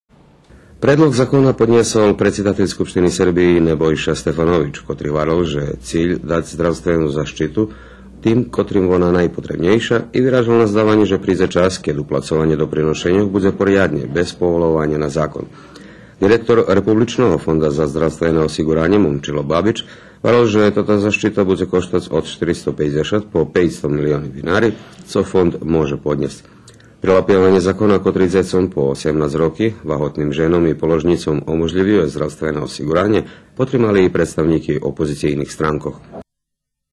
Осиґуранє за шицки ваготни жени, положнїци и дзеци/Osiguranje
Скупштински одбор за здравє и фамелию єдногласно потримал Предлог закона о защити дзецох, ваготних женох и положнїцох з котрим ше предвидзує же тоти ґрупи жительства буду мац здравствену защиту без огляду на тото чи сим оверени здравствени кнїжочки.